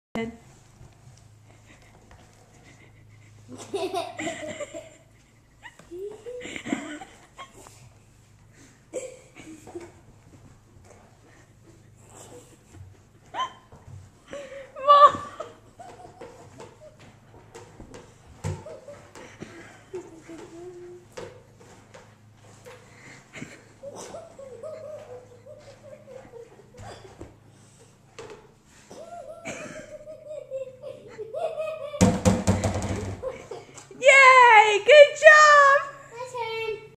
Brother In A Bucket!
This is my silly little brother being funny! :D